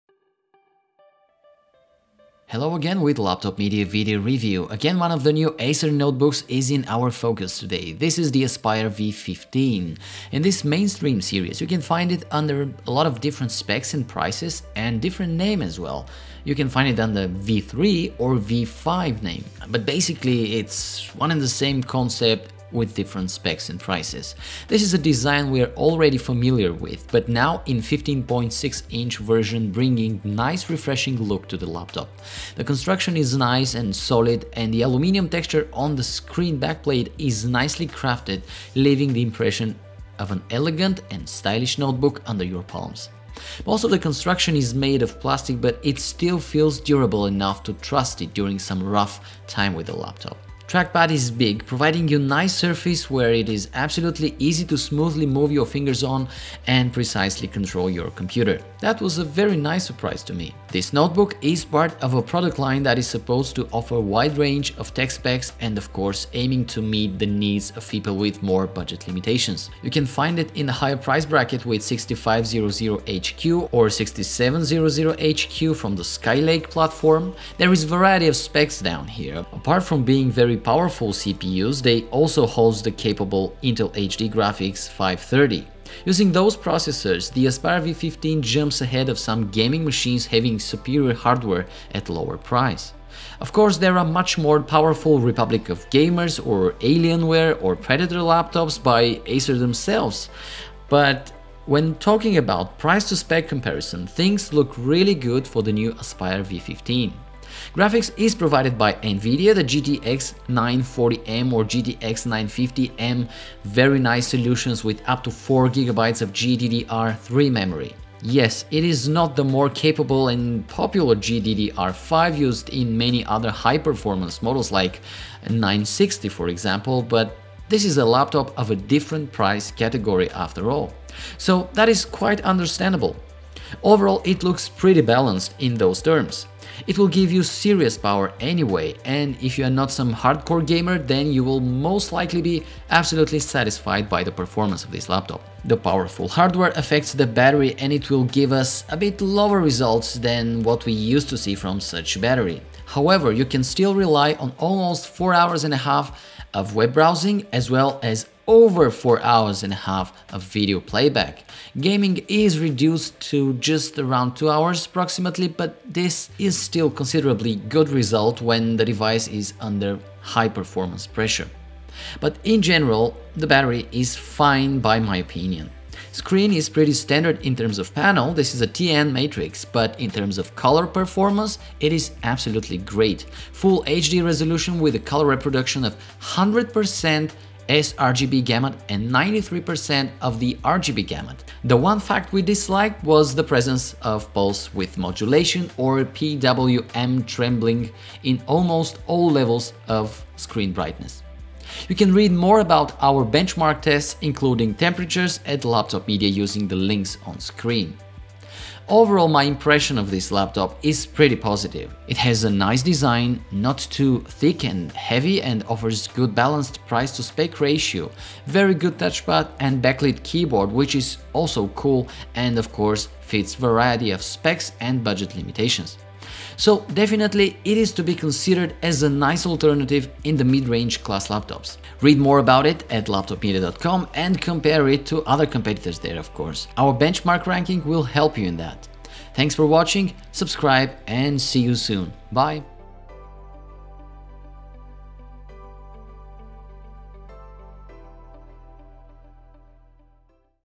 Acer Aspire V15 (V3-575, V5-591) video review
Inside Acer Aspire V15 (V3-575G) – disassembly, internal photos and upgrade options
Inside Acer Aspire V15 (V5-591G) – disassembly, internal photos and upgrade options